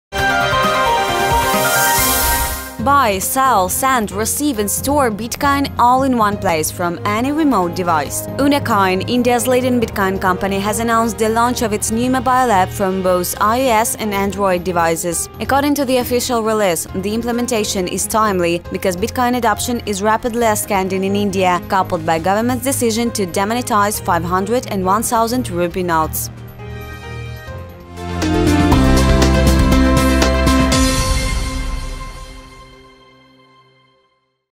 KCN: UnoCoin to launch app on IOS&Android
Buy, sell, send, receive and store bitcoin all in one place, from any remote device. Unocoin,  India’s leading Bitcoin company, has announced the launch of its new mobile app for both iOS and Android devices. According to the official release, the implementation is timely because bitcoin adoption is rapidly ascending in India, coupled by government’s decision to demonetize 500 and 1000 Rupee notes.
EDR: anna-profit
BTC: 1JdRXQuqicwSyn61cd9jyXzAk2QUfu4KEW